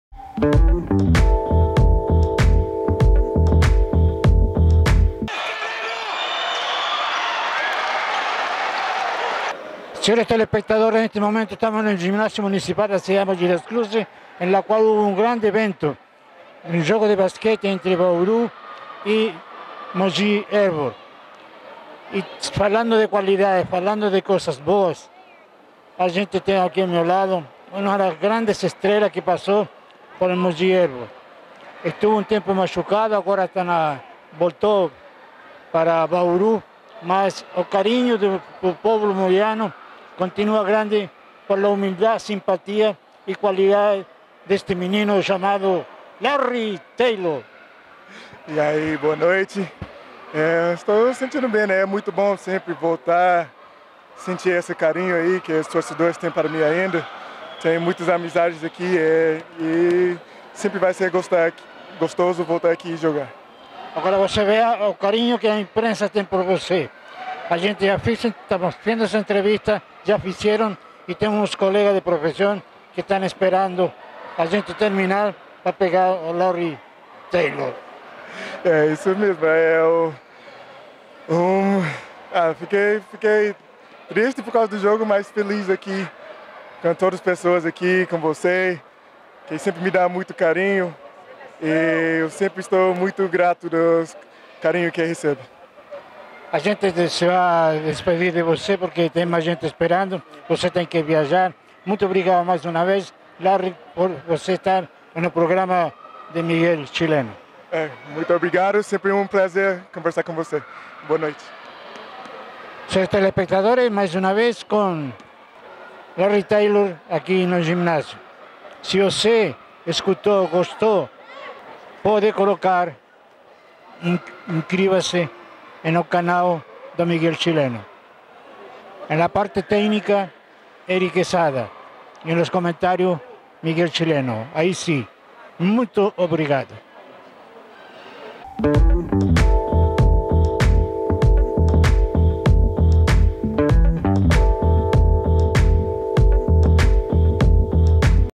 LARRY TAYLOR - Armador do Bauru Basket [Entrevista 15/11/2018]
Miguel Chileno em uma rápida conversa com Larry Taylor, após o jogo do Mogi/ Helbor contra a equipe do Sendi/ Bauru Basket , no ginásio Prof. Hugo Ramos, em Mogi das Cruzes.